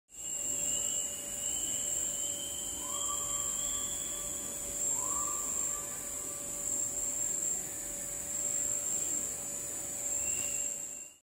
Raubfliege mit erbeutetem Mistkäfer im Regenwald bei Suckau, Sabah, Borneo, robber fly
Raubfliege mit erbeutetem Mistkäfer im Regenwald bei Suckau, Sabah, Borneo